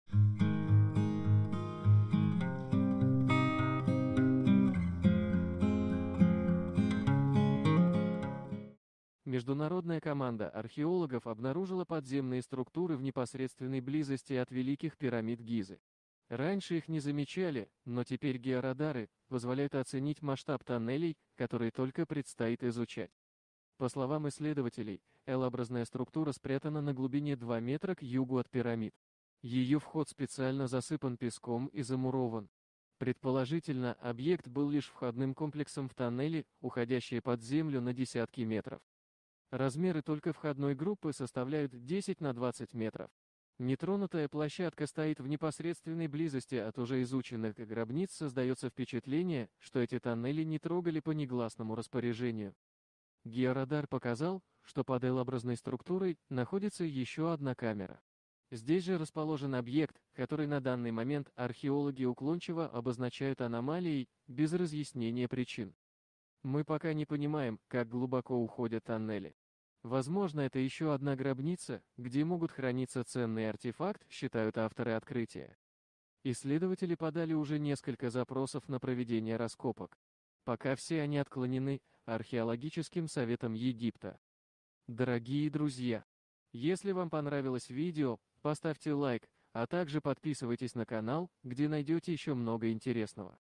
Ученые просканировали территорию вокруг Пирамид и обнаружили ужасное!
Международная команда археологов обнаружила подземные структуры в непосредственной близости от Великих пирамид Гизы. Раньше их не замечали, но теперь георадары позволяют оценить масштаб тоннелей, которые только предстоит изучать.